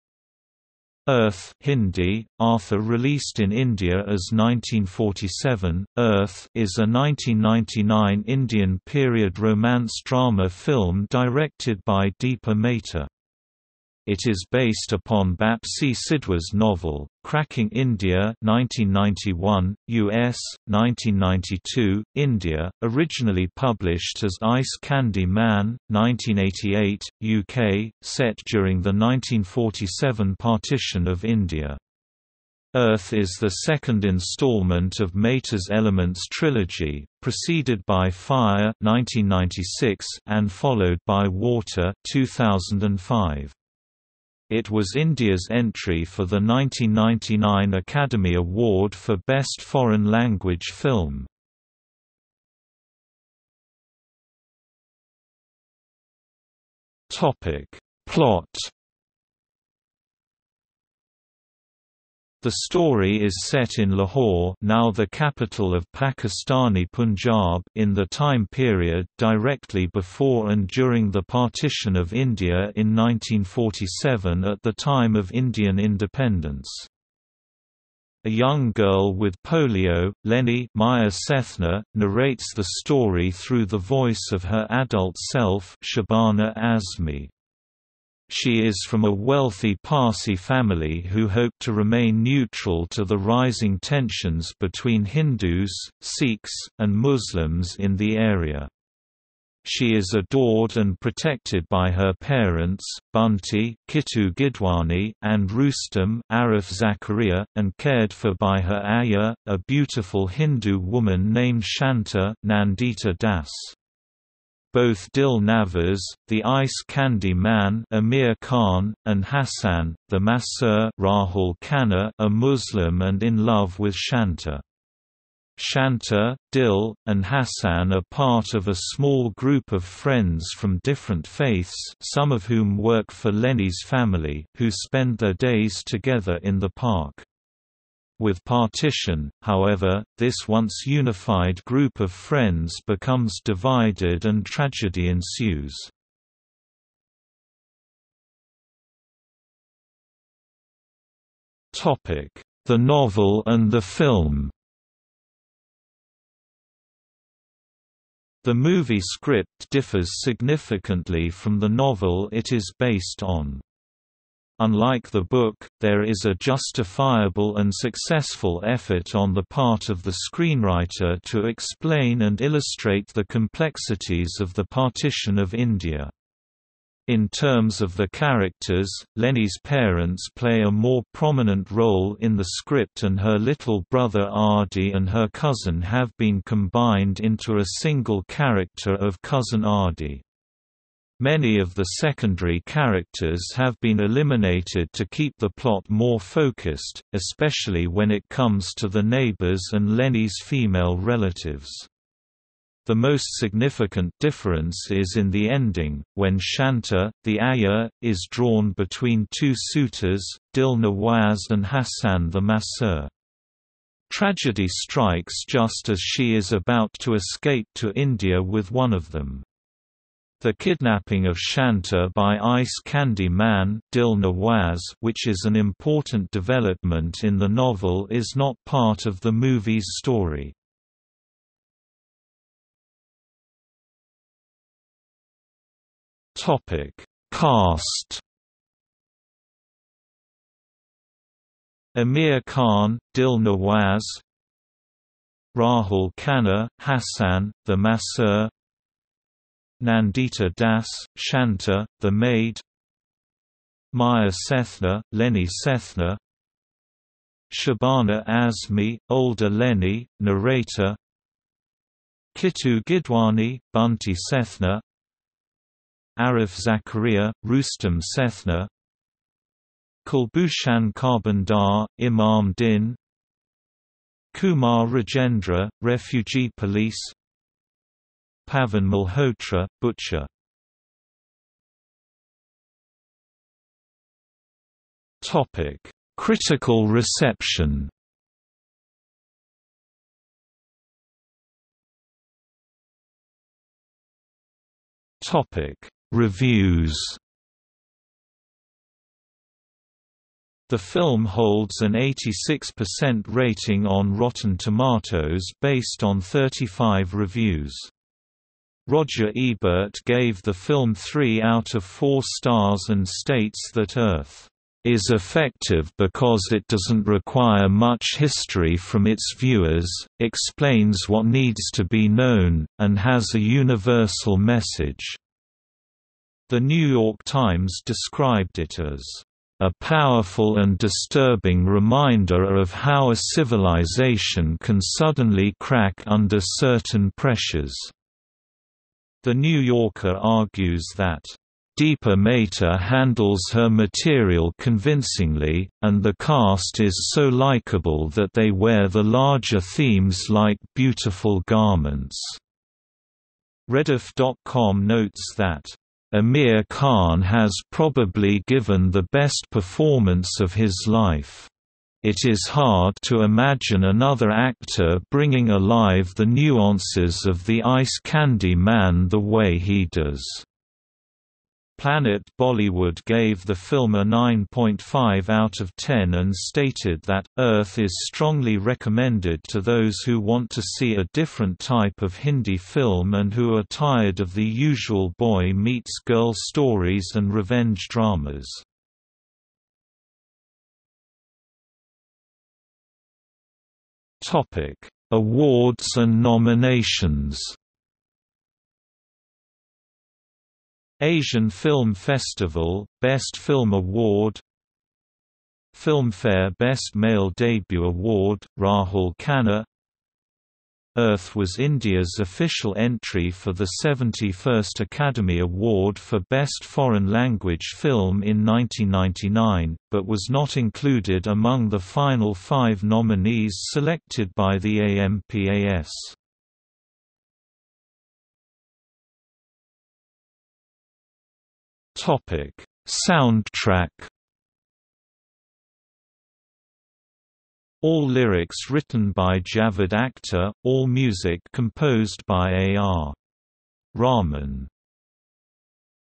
Earth (1998 film) | Wikipedia audio article
00:00:53 1 Plot
00:02:28 2 The Novel and the Film
00:03:52 3 Cast
00:04:41 4 Critical reception
00:04:51 4.1 Reviews
00:06:30 4.2 Awards and nominations
00:07:08 5 Soundtrack

- Socrates

SUMMARY
Earth (Hindi: अर्थ; released in India as 1947: Earth) is a 1999 Indian period romance drama film directed by Deepa Mehta. It is based upon Bapsi Sidhwa's novel, Cracking India (1991, US; 1992, India; originally published as Ice Candy Man, 1988, UK), set during the 1947 partition of India. Earth is the second installment of Mehta's Elements trilogy, preceded by Fire (1996) and followed by Water (2005). It was India's entry for the 1999 Academy Award for Best Foreign Language Film.